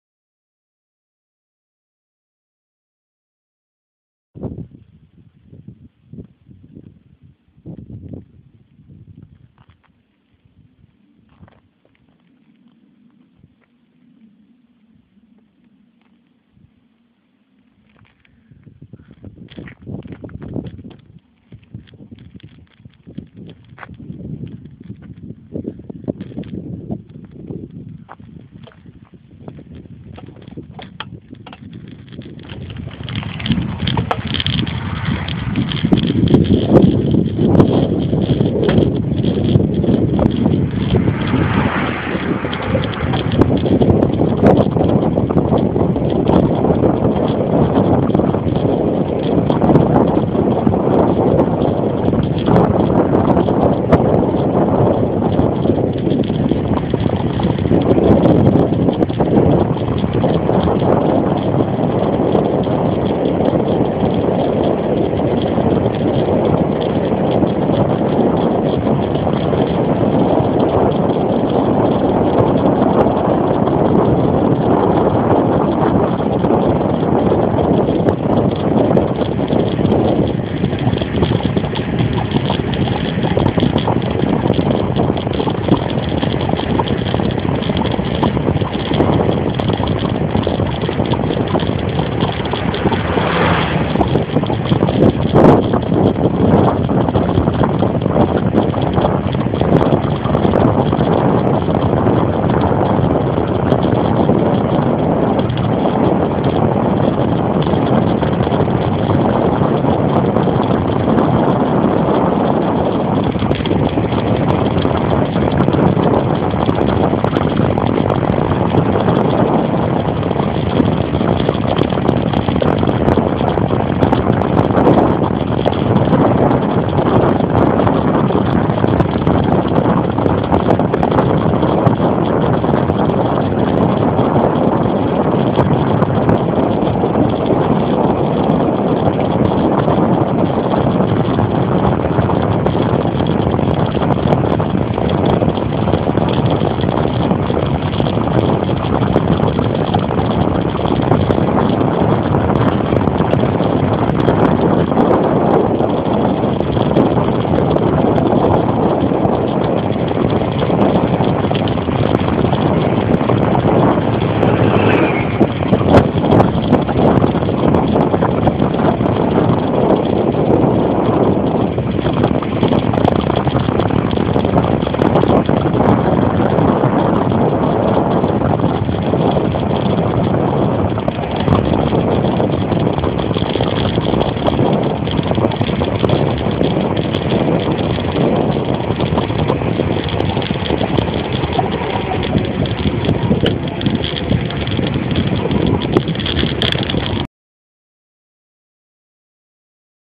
Cesta z Vysokého poľa do Domaník
Cesta z Vysokého poľa do Domaník z pohľadu bicykla.